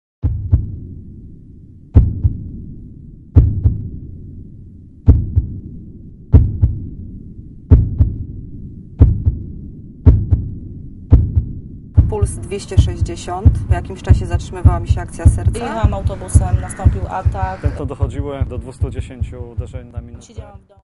L' ABLAZIONE DI ARITMIA - OPPORTUNITA' PER IL RITMO CARDIACO REGOLARE
A way of recovering from arrhythmia / Una via di guarigione dall'aritmia
(Impression 2 Films s.c. Sebastian Stec, Maciej Bochniak - POLAND)